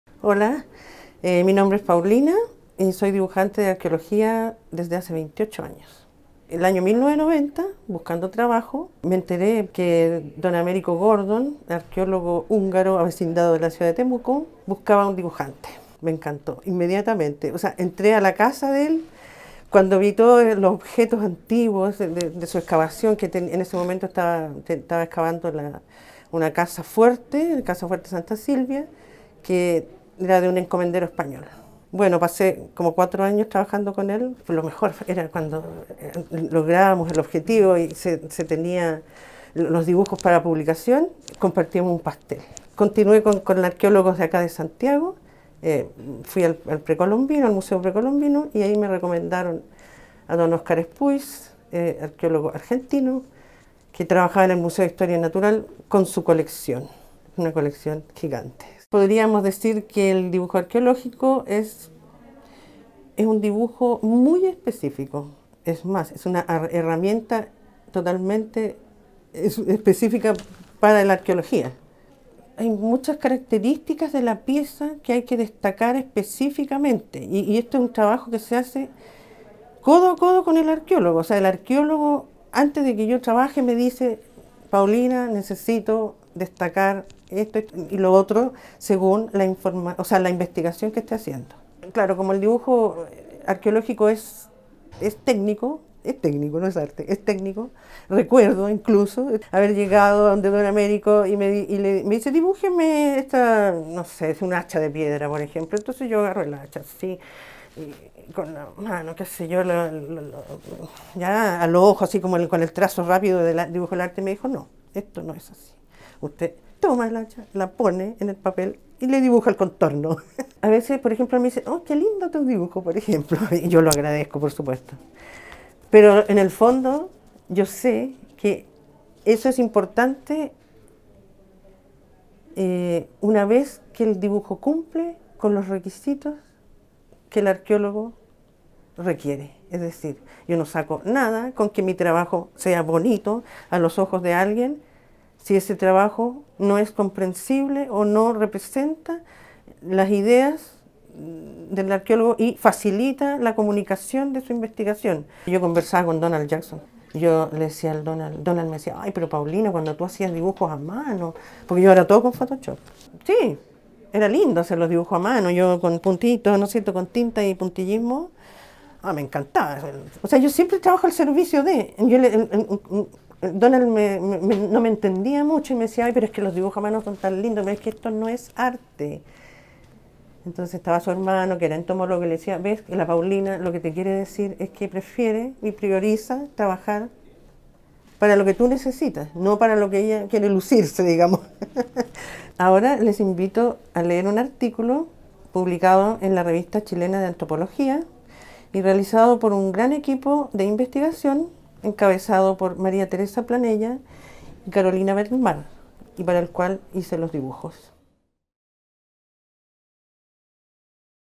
Paulina Chávez. El dibujo arqueológico
La dibujante Paulina Chávez describe el dibujo arqueológico y el trabajo con los arqueólogos para comunicar sus investigaciones con ilustraciones técnicas de diversos objetos científicos de la cultura material.

Filmado en el Departamento de Antropología de la Universidad de Chile en agosto de 2018 por Francisco Osorio.